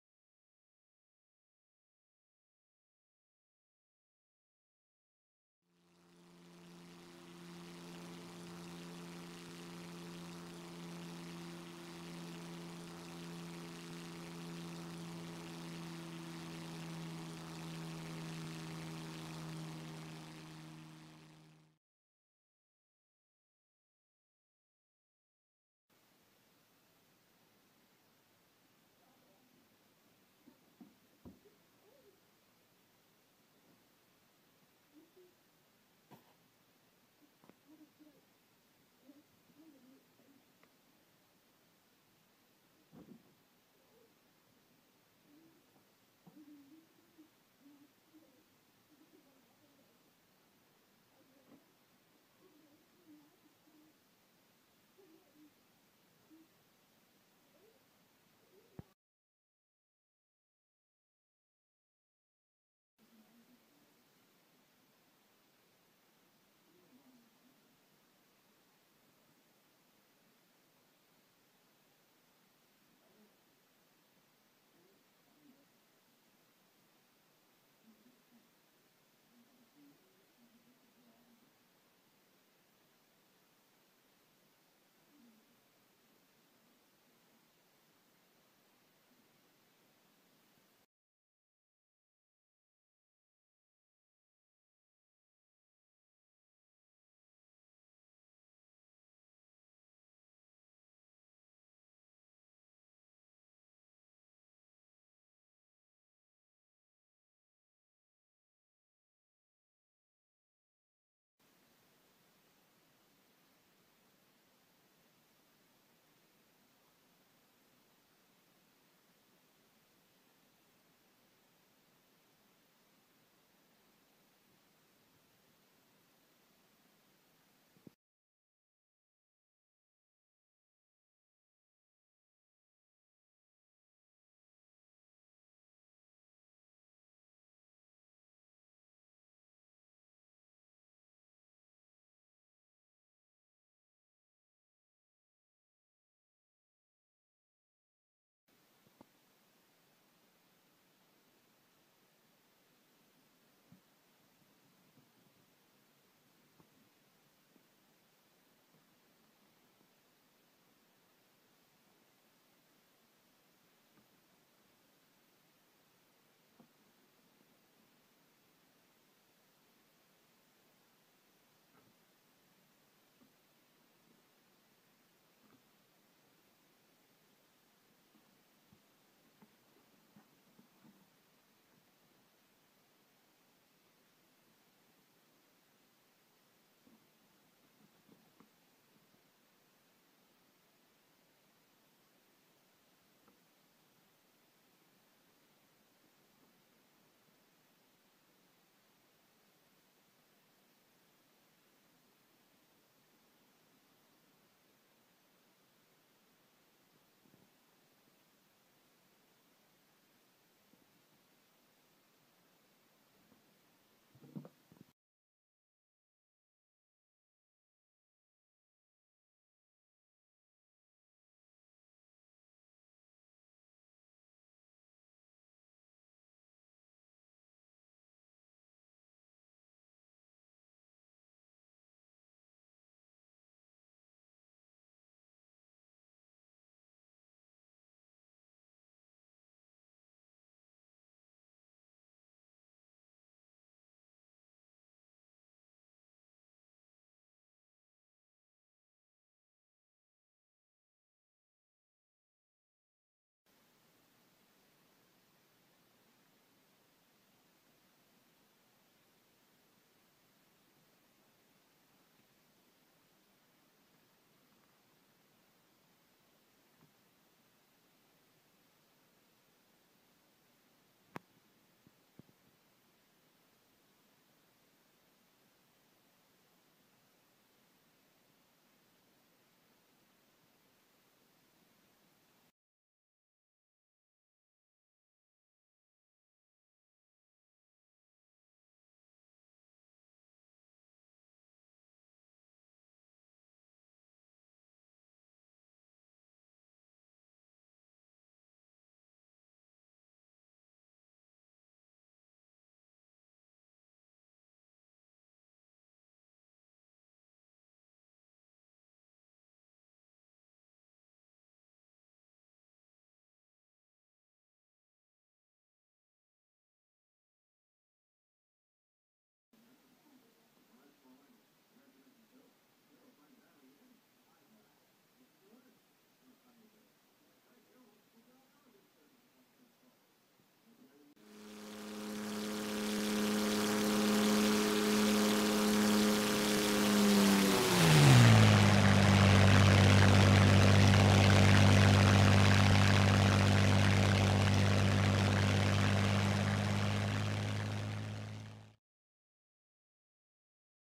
Tamiya 1/32 P-51D Build Part 1
The Packard Merlin 1650 engine with supercharger is built.  The instrumental panel is assembled and the seat with seat harness put together.